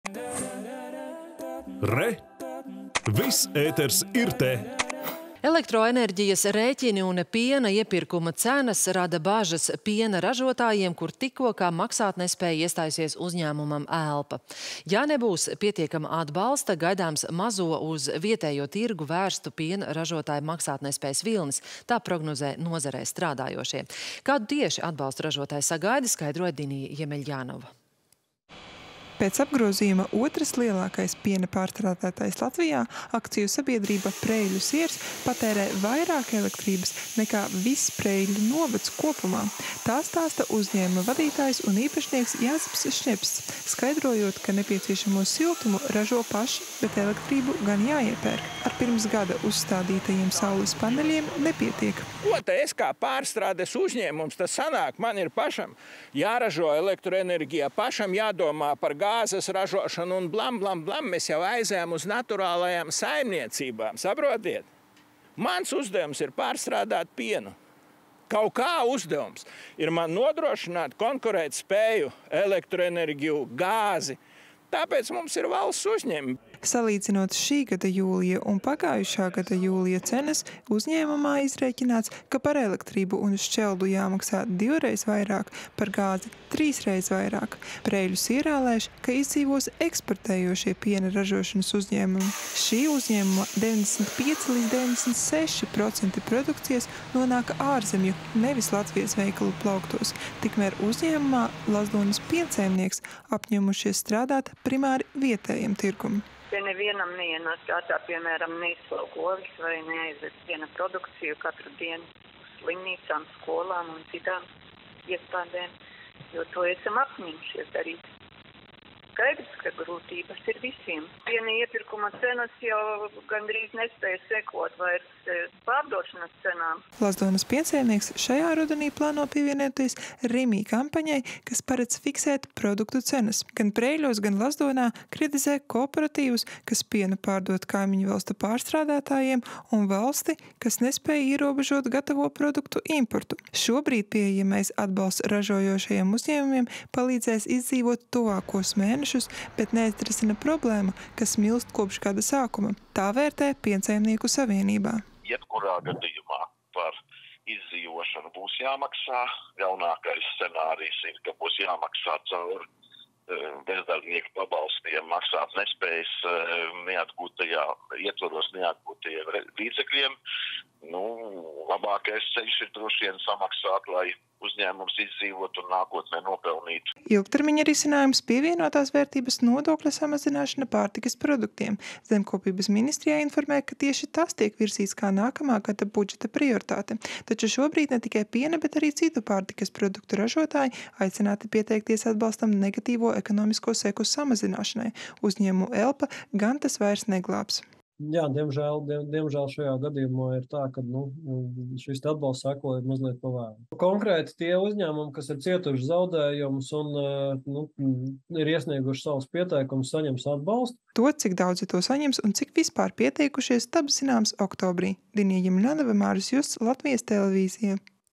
Energoresursu cenas gremdē piena ražotājus
Elektroenerģijas rēķini un piena iepirkuma cenas  rada bažas piena ražotājiem, kur tikko kā maksātnespēja iestājusies uzņēmumam “Elpa”.  Ja nebūs pietiekama atbalsta,  gaidāms mazo, uz vietējo tirgu vērstu piena ražotāju maksātnespējas vilnis, tā prognozē nozarē strādājošie. Kāda tieši, skaidroja Dinija Jemeļjanova.